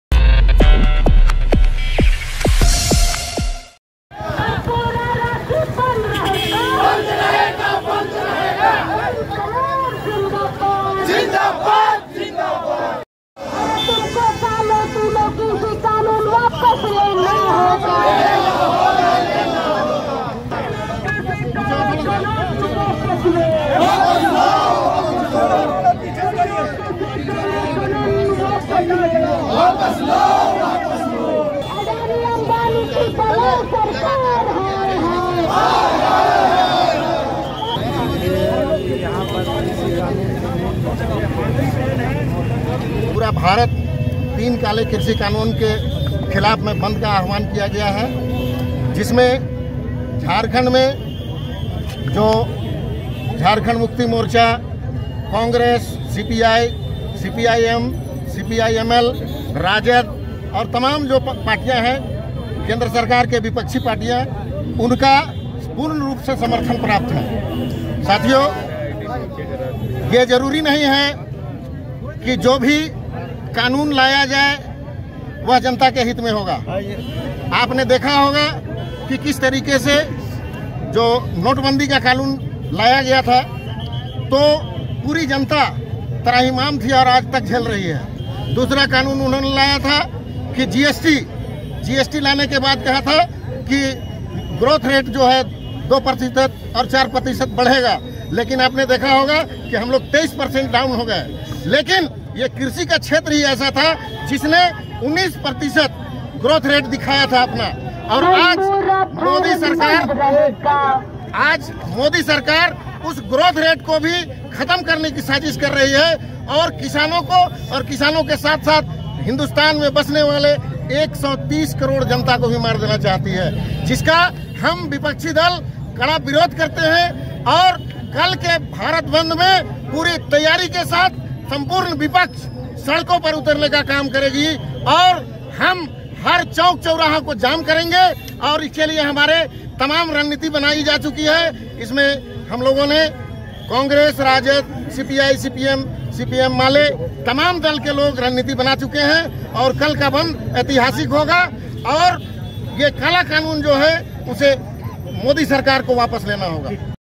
27 सितंबर, भारत बंद के पूर्व संध्या पर राजधानी रांची में निकाला गया मशाल जुलूस....
राँची: अखिल भारतीय किसान संघर्ष मोर्चा के द्वारा 27 सितंबर को भारत बंद के पूर्व संध्या पर राँची में सैनिक बाजार मार्केट से अल्बर्ट एक्का चौक तक मशाल जुलूस निकाला गया। इस मशाल जुलुस में कांग्रेस, झमुमो, राजद के साथ सभी वामदल के लोग शामिल हुएं।
मौके पर झारखंड मुक्ति मोर्चा के जिलाध्यक्ष, मुस्ताक अहमद ने केंद्र सरकार की जमकर आलोचना करते हुए कहा कि, केंद्र सरकार द्वारा किसी भी तरह का कानून लाया जाता है तो ये जरूरी नहीं कि सभी कानून जनता के हित में हो। केंद्र की मोदी सरकार के द्वारा लाए गए तीनों कृषि कानून किसानों के हित में नहीं है और कल के भारत बंद को ऐतिहासिक बनाने के लिए झारखंड में झामुमो, राजद, कांग्रेस और वामदलों ने रणनीति बना ली है। कल का भारत बंद ऐतिहासिक बंद साबित होगा। केंद्र सरकार को यह तीनों कृषि कानून वापस लेना ही होगा।